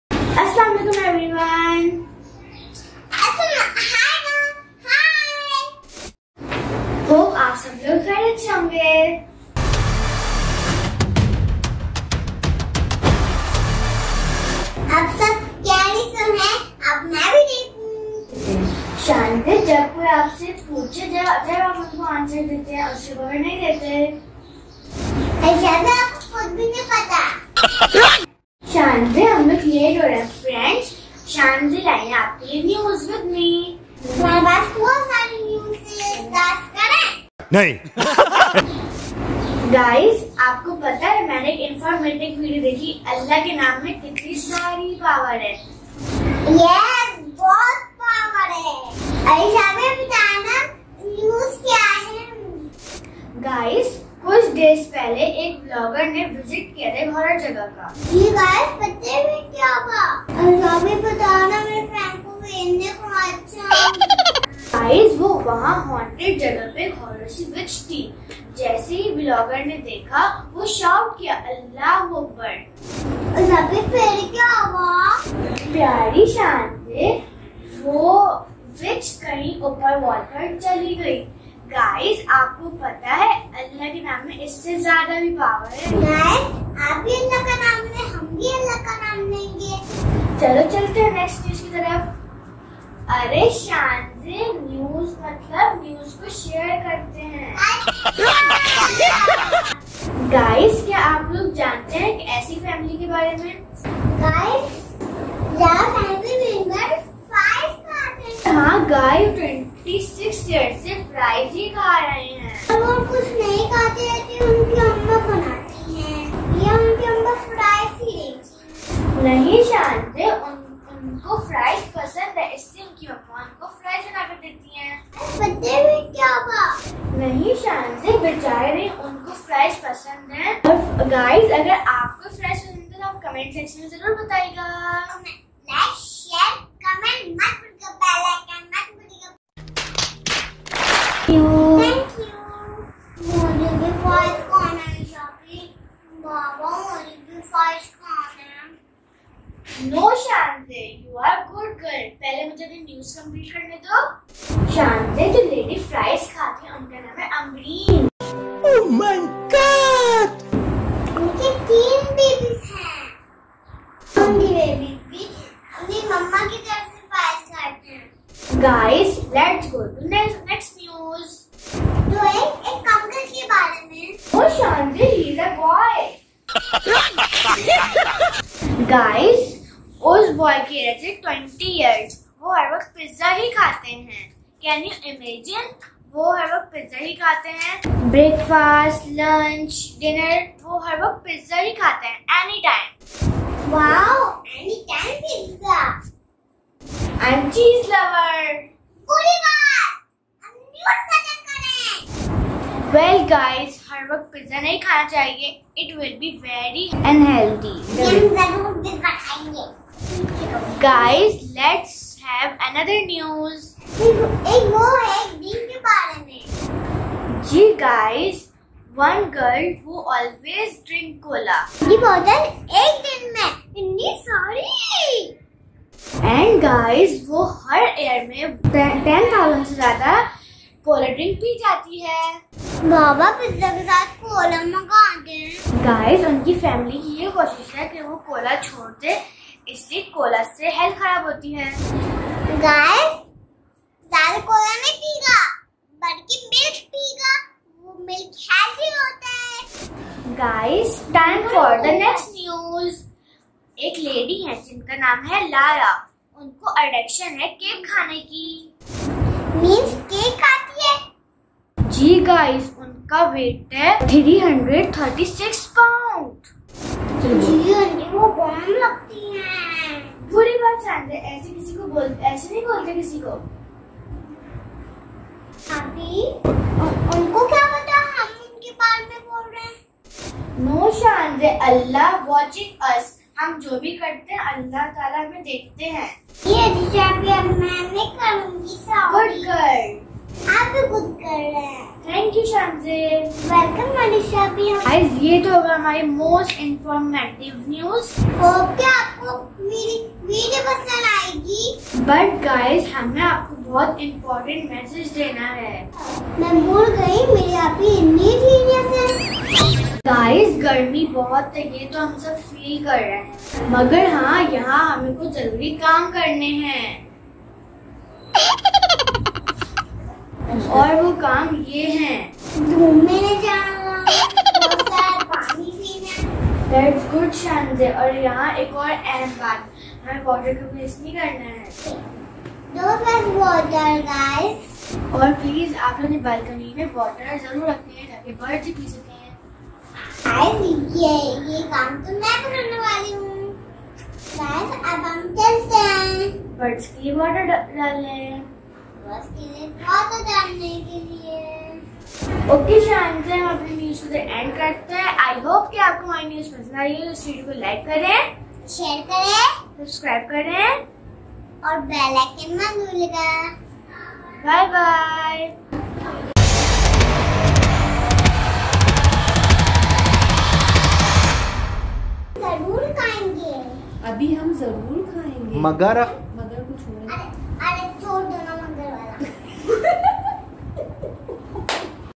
Areesh & Shanzay || about People's Food Informative News Vlog Masti Fun There also Bloopers
In this video, you will watch about People's food and Power of ALLAH by Areesh & Shanzay.

I Hope you enjoy this video as much as we enjoyed

happy family show
If you like our videos please share.